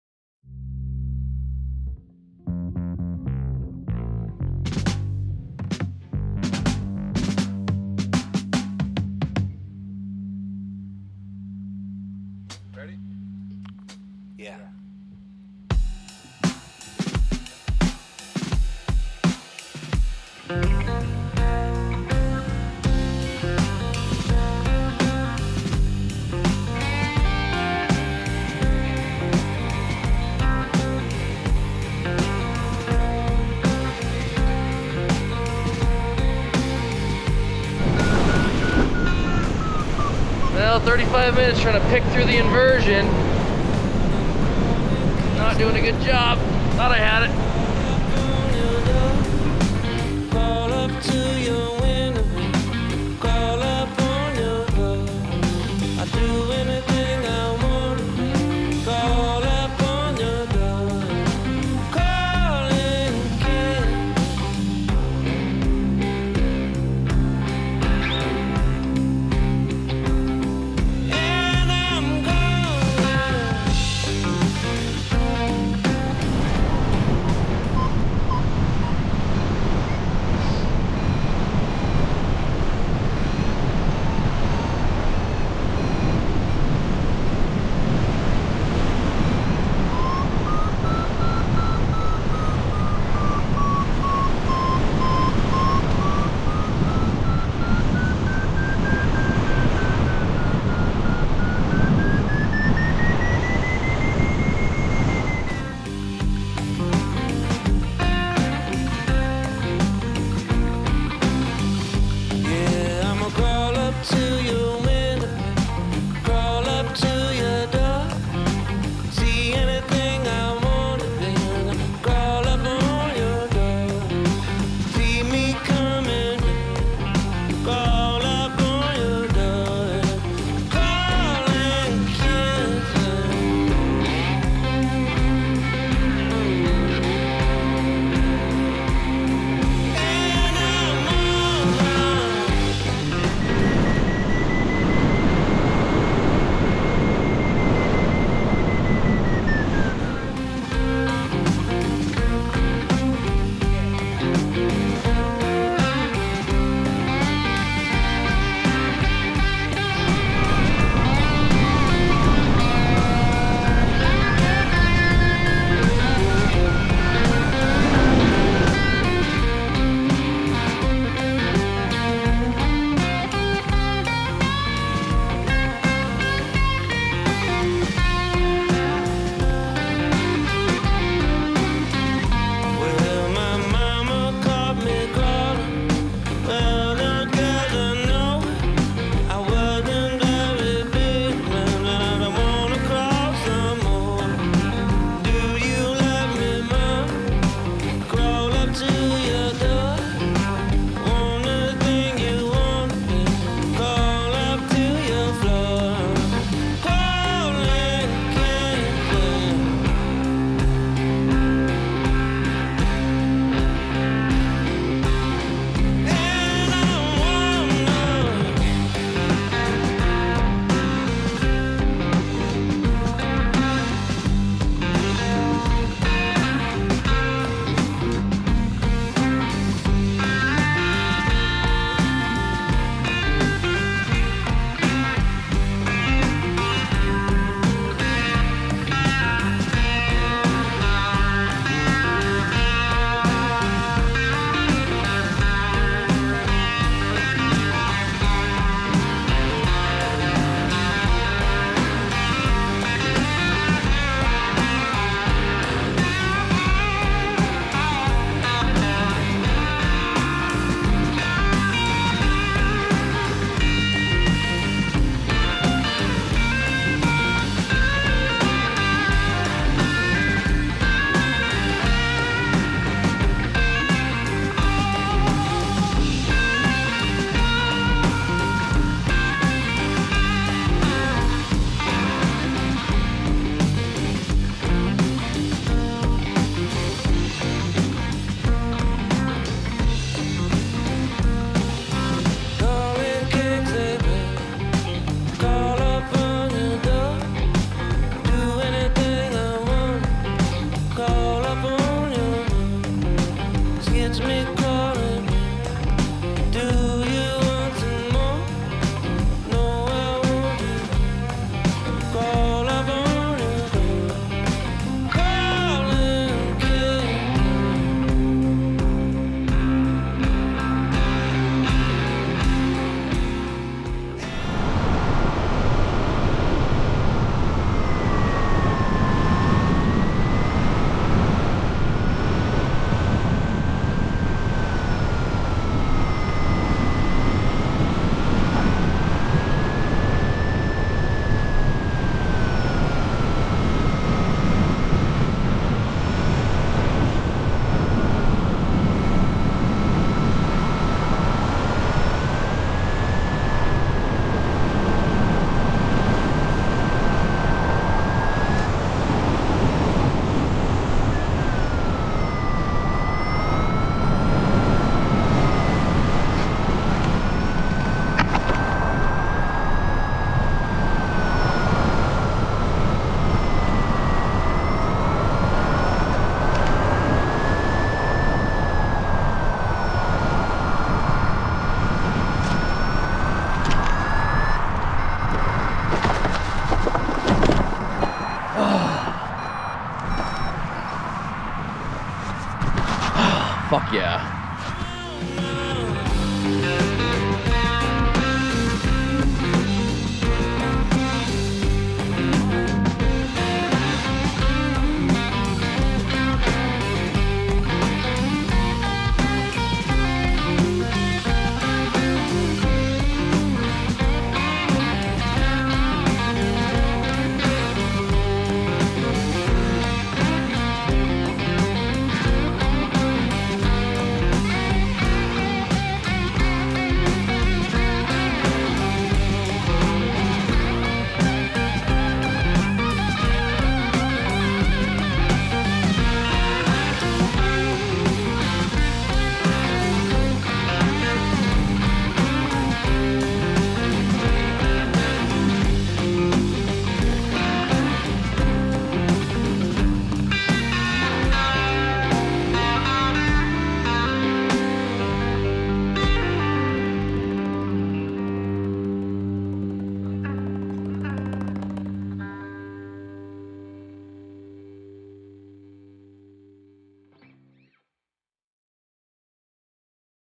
4-28/21 Tekoa WA to Cocolalla Lake ID - Atos VQR - 4K
Fun flight.  Thought I was landing at the Athol airport but caught a climb to keep the flight going.